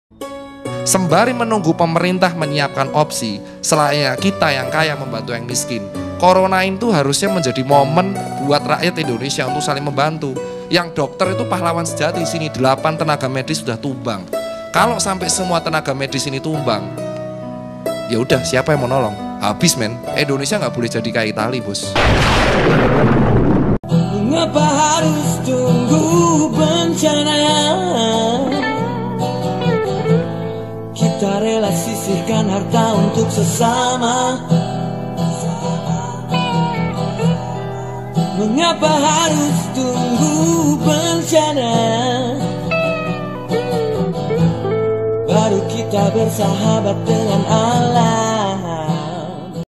Story'wa dokter Tirta selayaknya Kita yang Kaya Bantu Miskin || Covid 19
Sekedar Quotes
Voice Dokter Tirta Mandiri Hudhi
Dokter Tirta mengusulkan ide tersebut sebagai langkah yang bisa dilakukan sembari menunggu pemerintah menyapkan opsi menangani virus corona.

"Corona itu harusnya menjadi momen rakyat Indonesia saling membantu,

Itu saja sekian dan terimakasih..